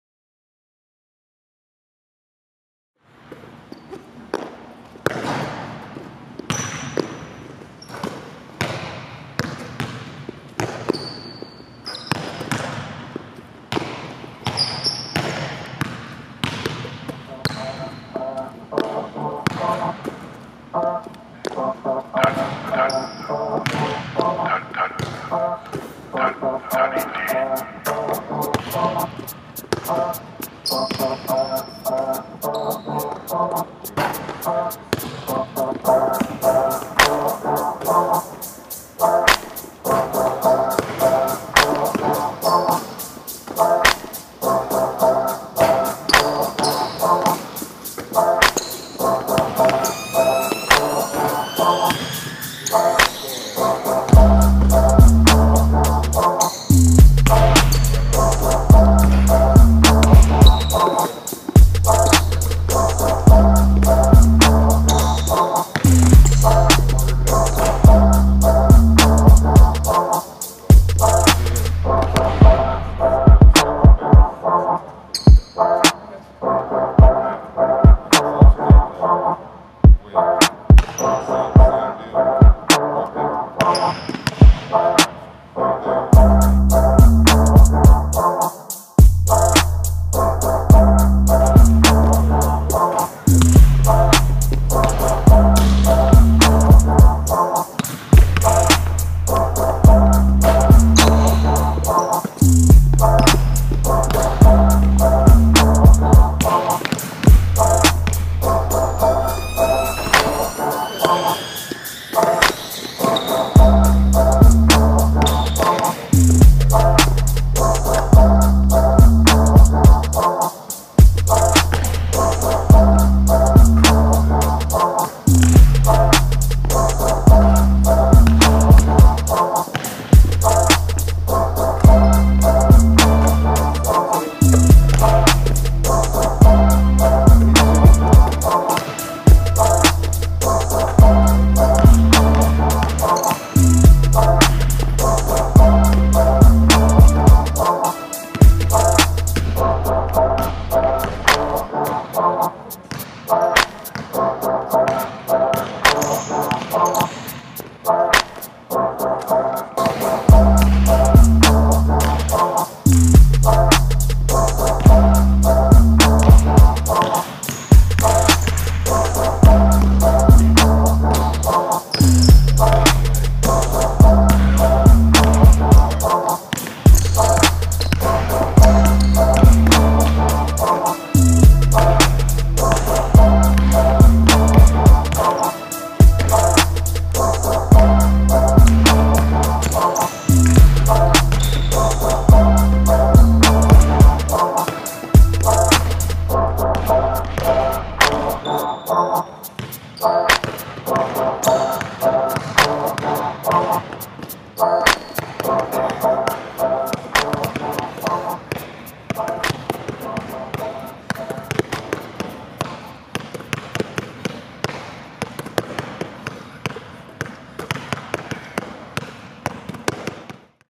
Brad Pinchback Basketball Player Development: Abby Meyers
Client Abby Meyers is one of the best 2017 high school basketball players in the country. The 6-0 Princeton commit was selected 1st team All Met by the Washington Post and Montgomery Sentinel Player of the Year. She led Walt Whitman HS to a MD State Championship as a junior and is ranked among ESPN's Top 60.